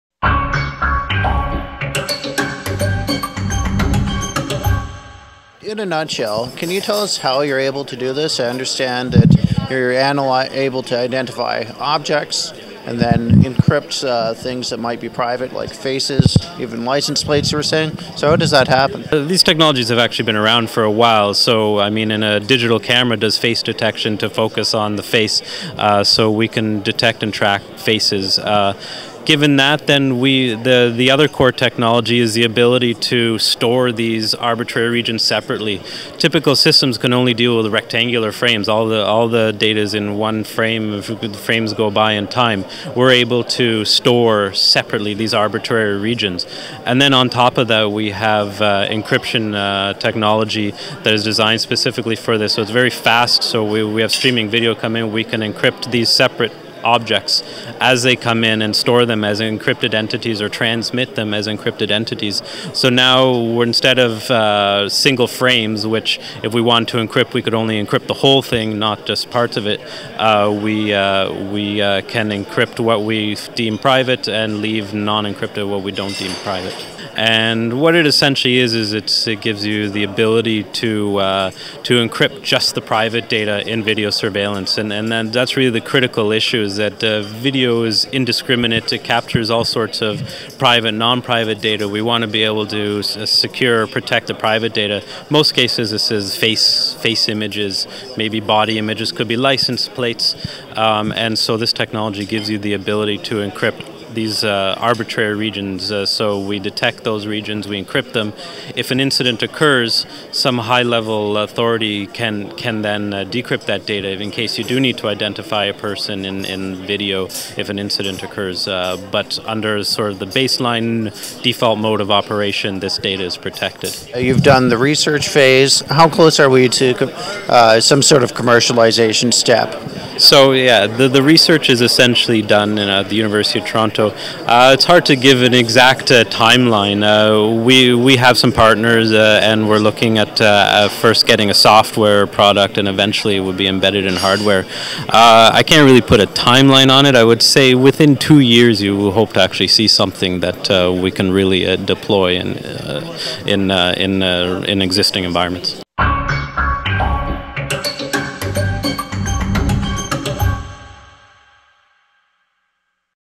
Video encryption system protects privacy on CCTV
Karl Martin has developed a system that allows individual's privacy to be protected from a video surveillance system. At the same time, authorities can still effectively use it for security purposes.